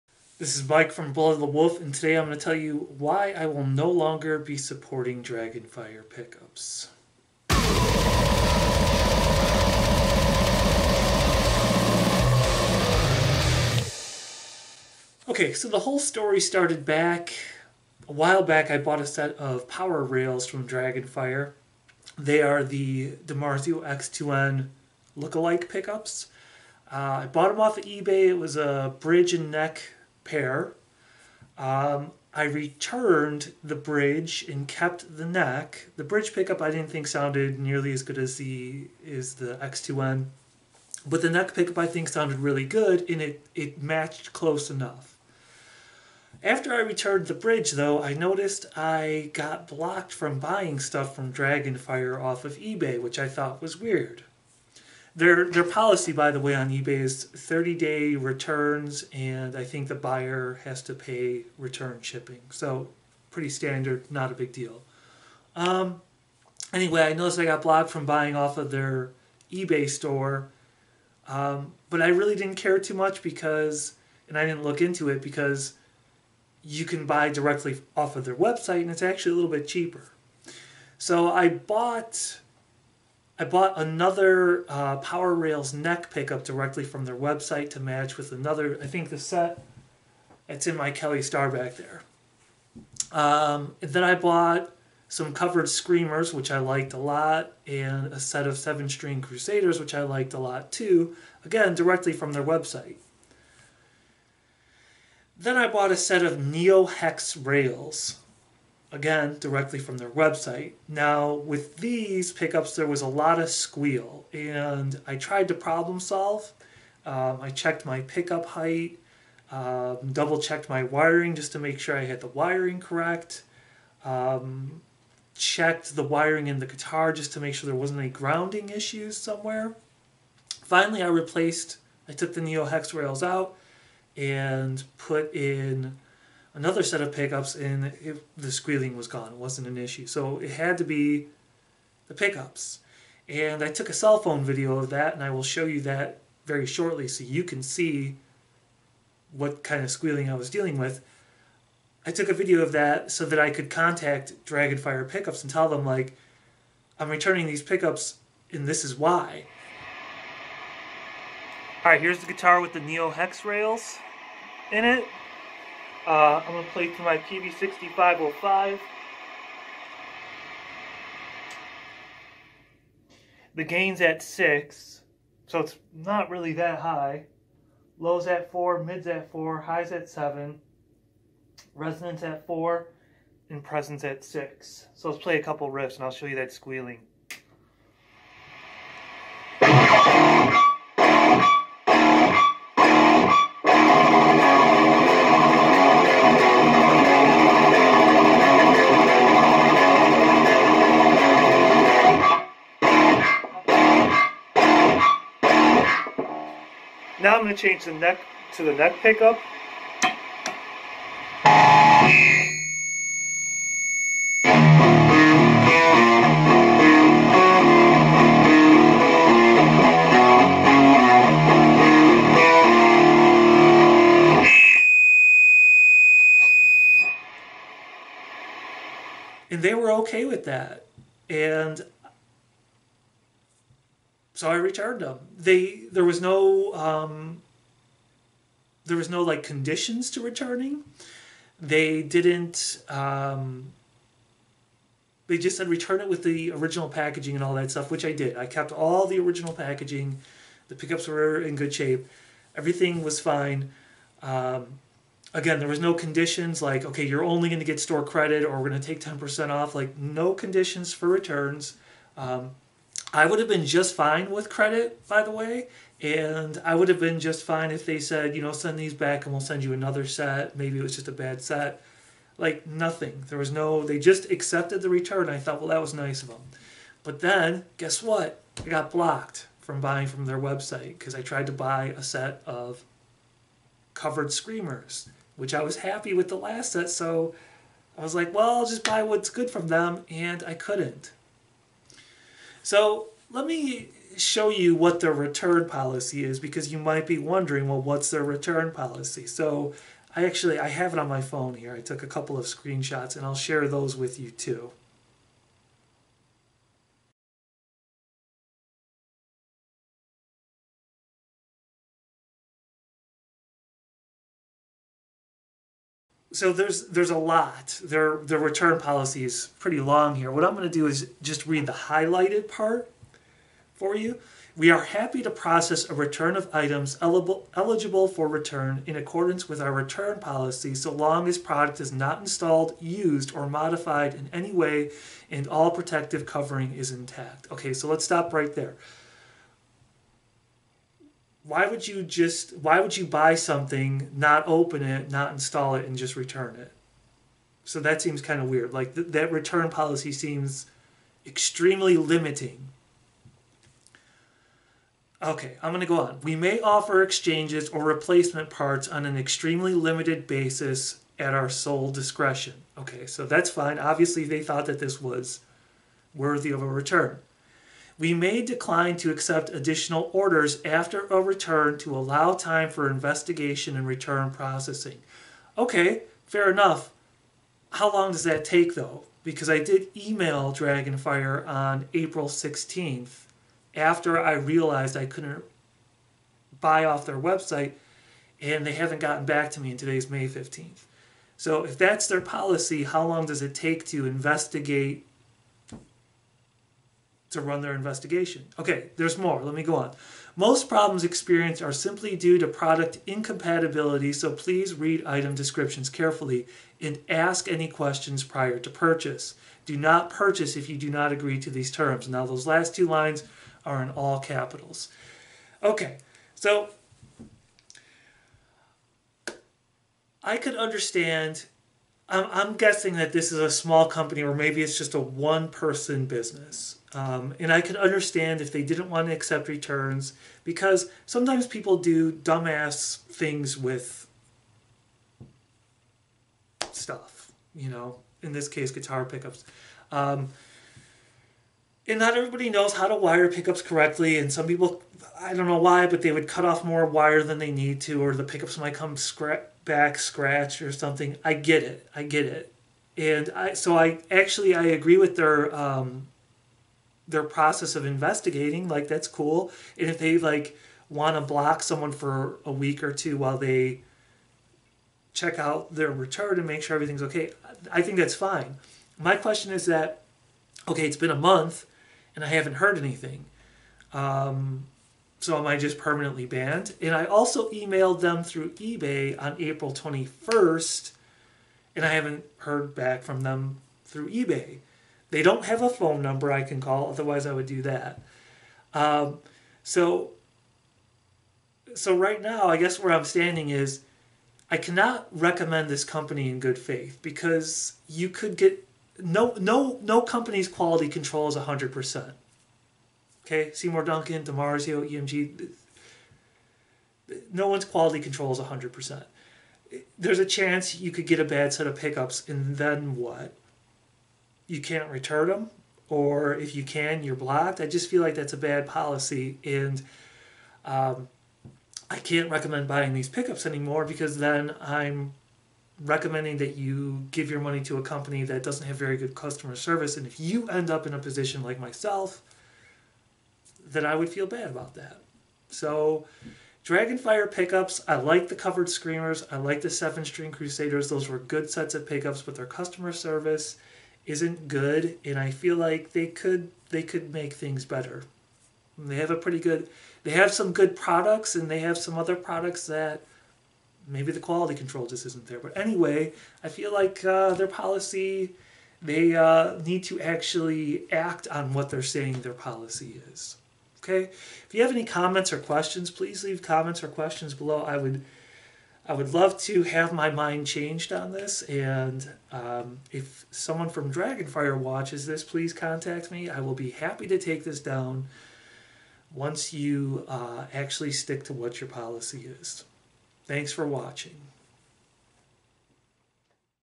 Dragonfire Pickups - Why I can no longer support them
In this video I explain my reasons for discontinuing my support of Dragonfire Pickups. I have purchased two of their Power Rails neck humbuckers, a set of covered Screamers, a set of Crusaders and a set of NeoHex Rails. I was a happy and returning customer. After returning some the squealing NeoHex Rails, I was "blocked" from their website and eBay store without explanation or response to my inquiry.